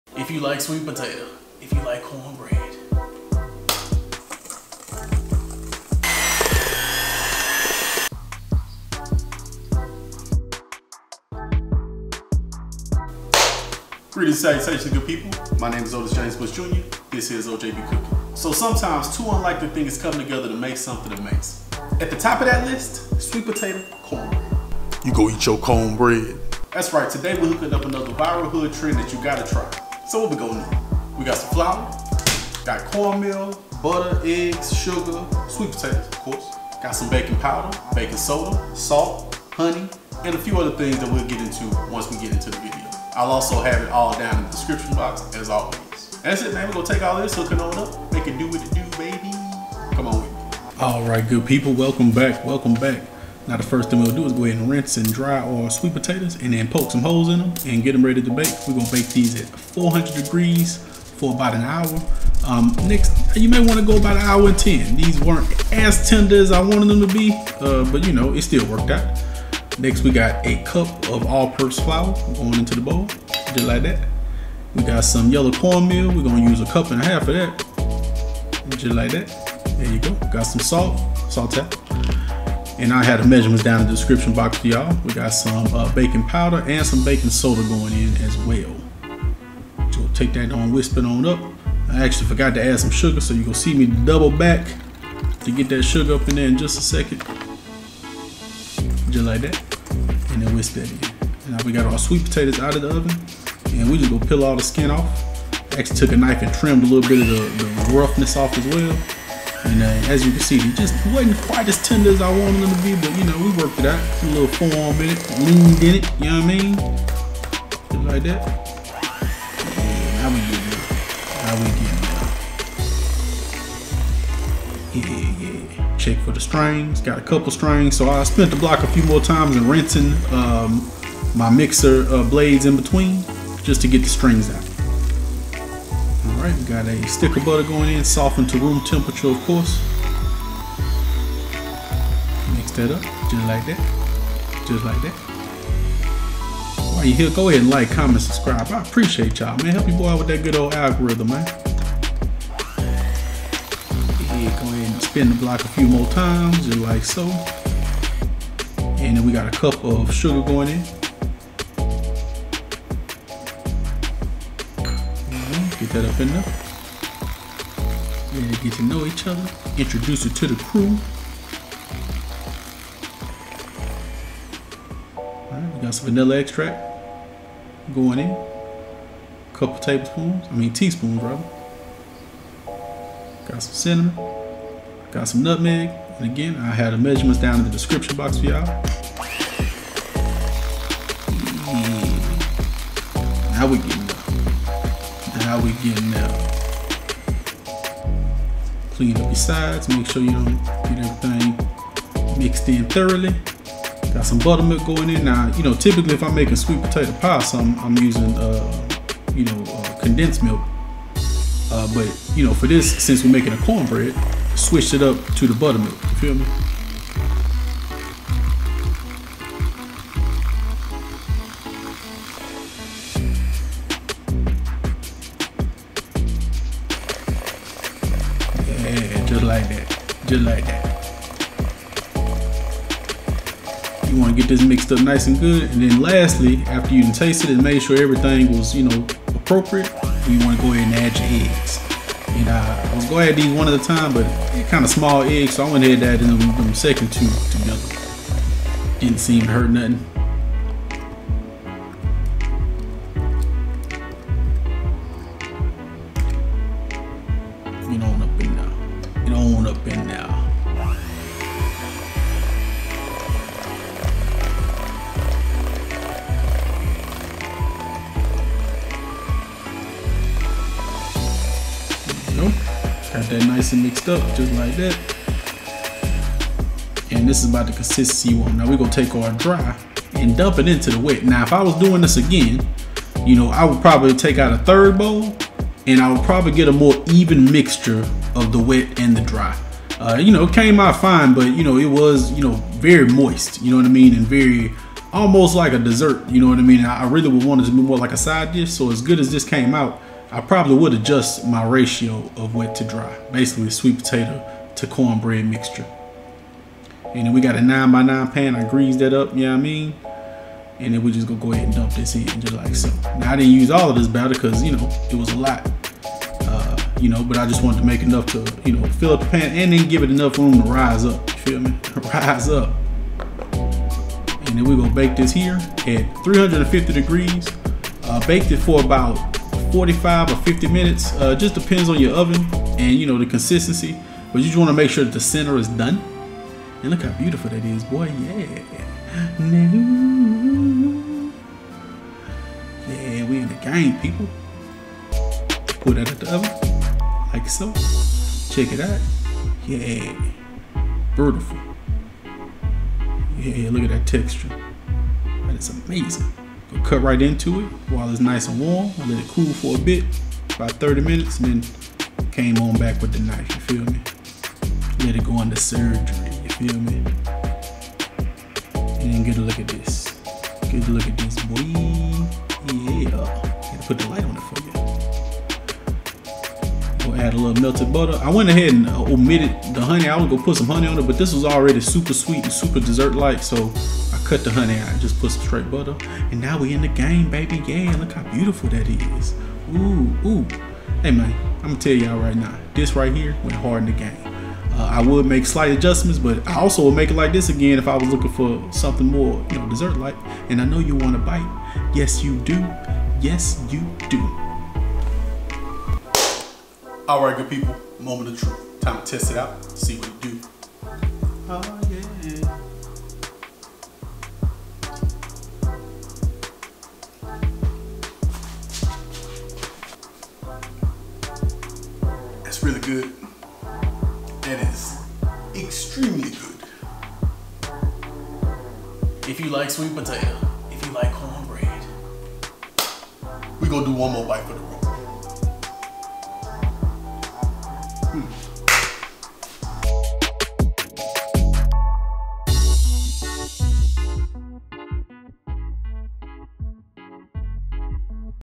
Best Sweet Potato Cornbread | Sweet Potato Cornbread Recipe | So Good
Greetings and Salutations good People! We are back with another soulfood fusion banger! Today, we are making Sweet Potato Cornbread! Thats right, we making another viral hit that makes you rethink food! Come on with me!

What you will need:
3 Med Sweet Potatoes
1 Stick of Softened Unsalted Butter
1 Cup Sugar
2 Cups Buttermilk
2 Large eggs (I used 3 because they were small)
1/2 Teaspoon Nutmeg
1 Teaspoon Cinnamon
2 Teaspoons Vanilla Extract
1 1/2 Cups Corn Meal
1 Cup All Purpose Flour
1/2 Teaspoon Salt
1 Tablespoon Baking Powder
1 Teaspoon Baking Soda
2 Tablespoons melted butter (for topping if desired)

Notes: Decided not to use the honey with the butter because this was pretty sweet already. For a a less sweet and more cornbread texture, adjust the mix of dry (cornbread mixture) and wet (sweet potato mixture). Baked Sweet Potatoes at 400 degrees for about an hour. I would go about 1 hour 15 mins to make sure they are super tender next time. Baked the cornbread mixture at 350 for about 50 mins until middle was done. Adjust as needed